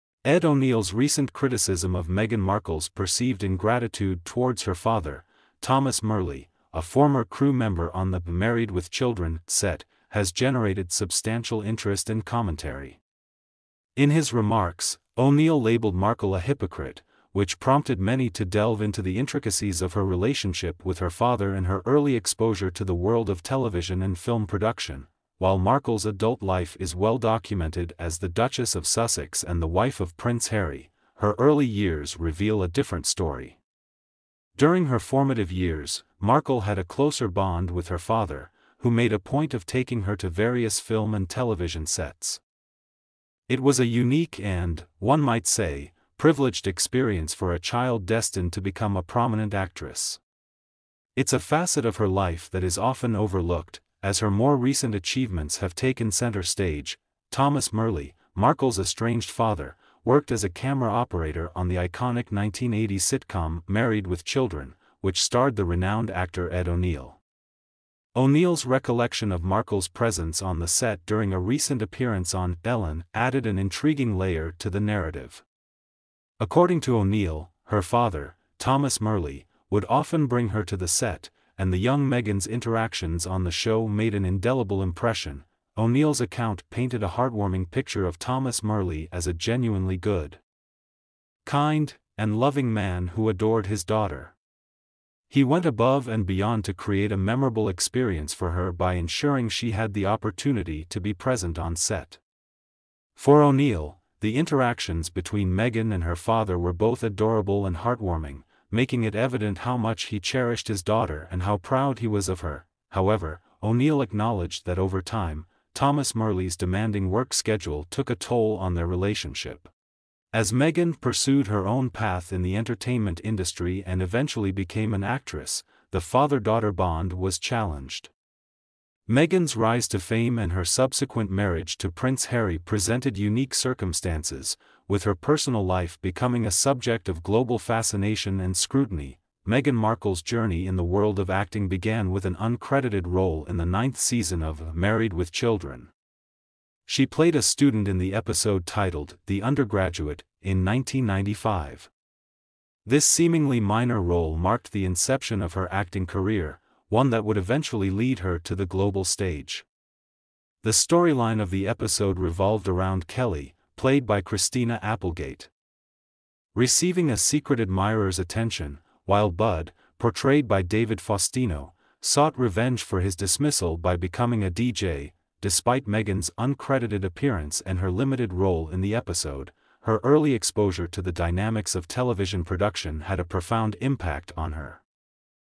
Ed O'Neill Blasted Meg for Her Disrespectful Behavior Towards her Father Thomas
Ed O'Neal's recent criticism of Megan Markle's perceived ingratitude towards her father, Thomas Marle, a former crew member on the "Married with Children" set, has generated substantial interest and commentary. In his remarks, O'Neal labeled Markle a hypocrite, which prompted many to delve into the intricacies of her relationship with her father and her early exposure to the world of television and film production.